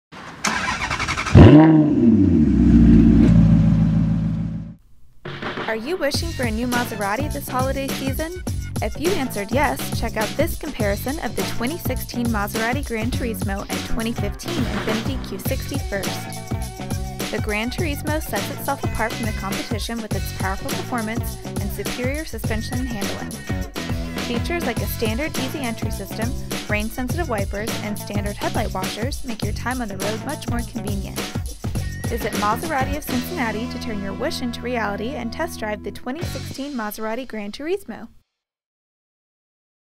2016 Maserati GranTurismo vs. 2015 Infiniti Q60
The 2016 Maserati GranTurismo and 2015 Infiniti Q60 show off their best features but the GranTurismo has an edge over the competition! Visit Maserati of Cincinnati to take a closer look at the 2016 Maserati GranTurismo!

4113 Plainville Rd.